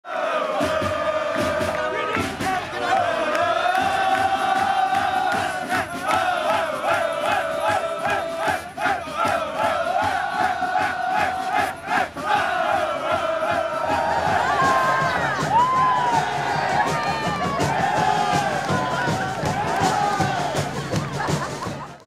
Retiring soccer player Juan Herndandez gets tossed into the air after 6-0 win in testimonial match
Chattanooga Football Club is a professional team playing in the third division of the U.S. Soccer pyramid. Check us out at chattanoogafc.com